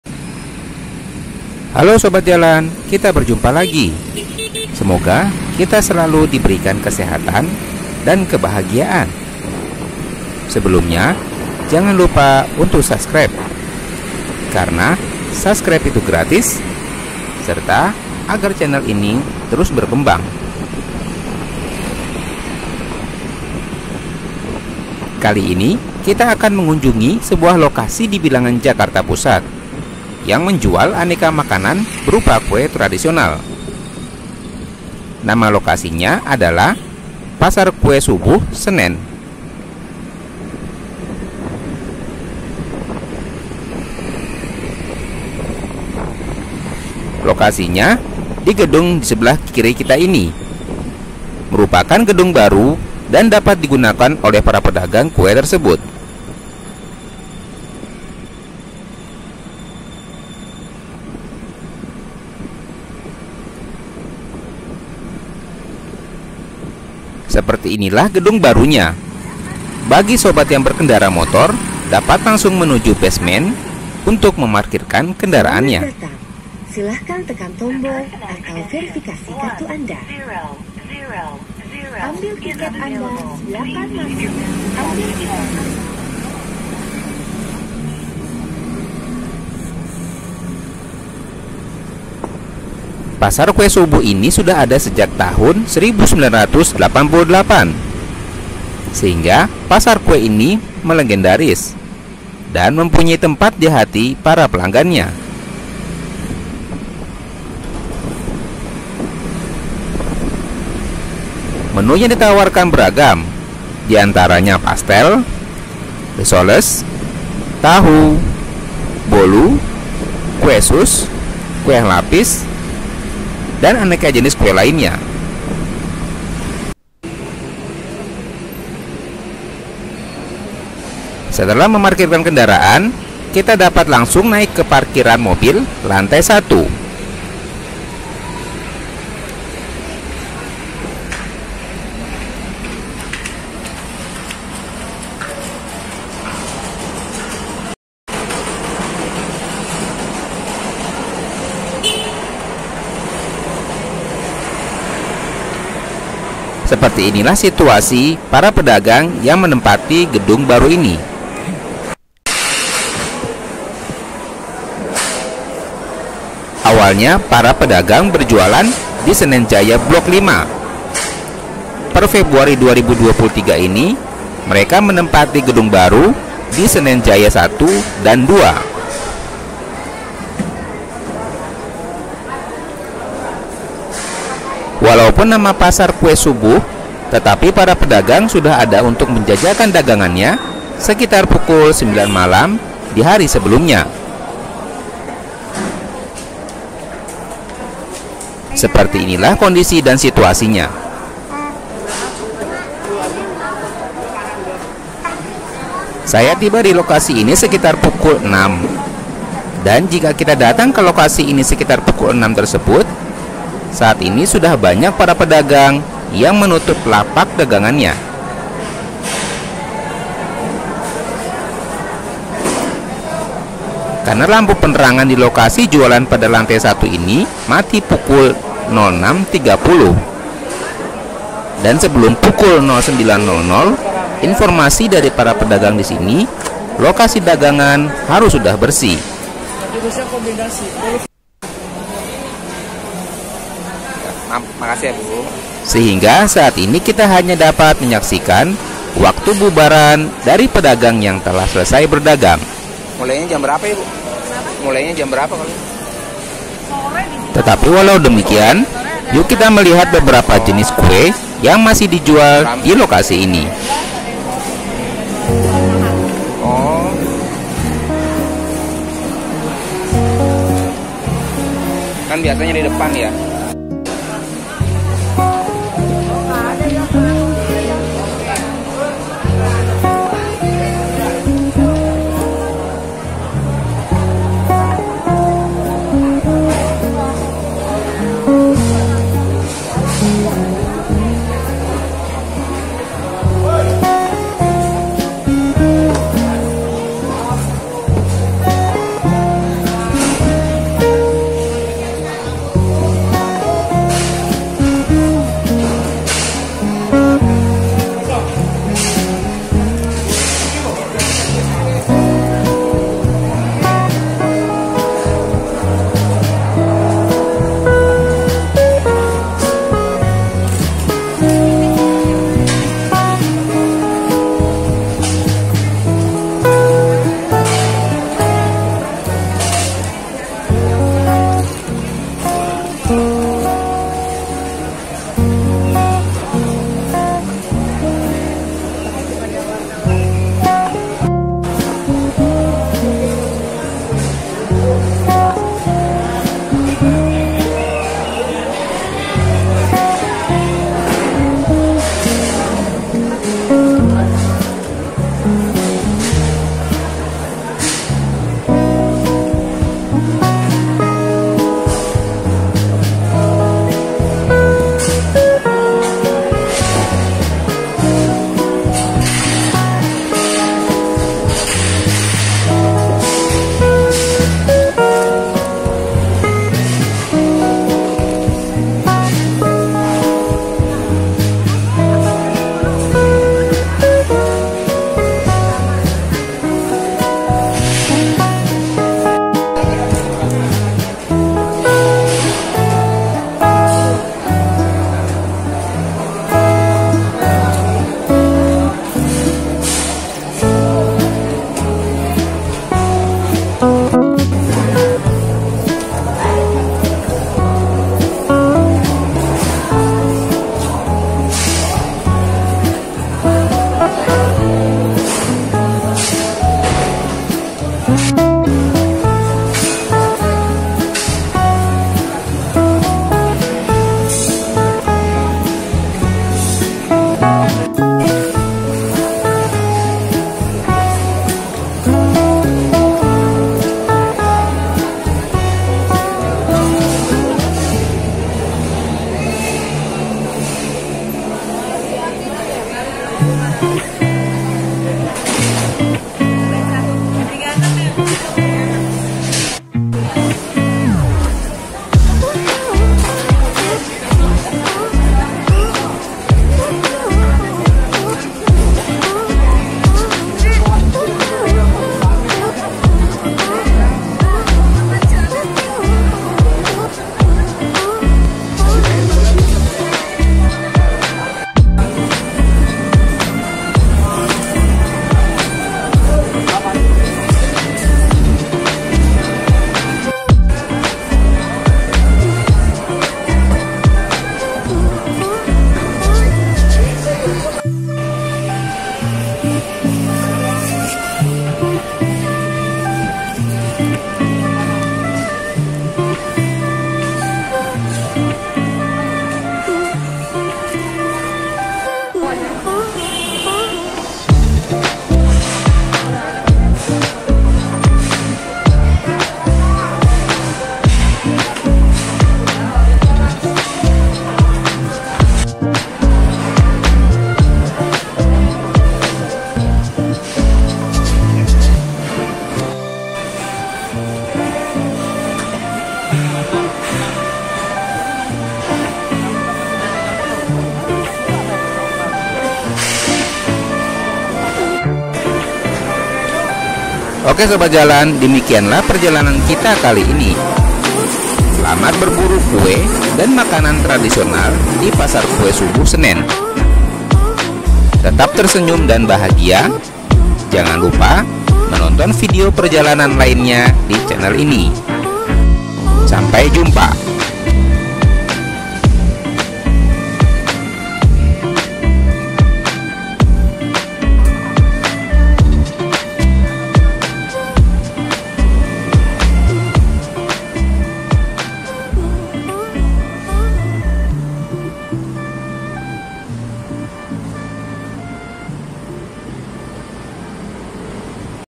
Pasar Kue Subuh Senen di lokasi baru, kondisinya pagi hari || Walking in Jakarta 🇮🇩
Kali ini kita akan mengunjungi sebuah lokasi di bilangan Jakarta Pusat yang menjual aneka makanan berupa kue tradisional.
Nama lokasinya adalah  Pasar Kue Subuh Senen.

Per bulan Februari 2023, para pedagang telah menempati lokasi baru yaitu di gedung Senen Jaya 1 dan 2, dimana sebelumnya menepati gedung Senen Jaya Blok 5.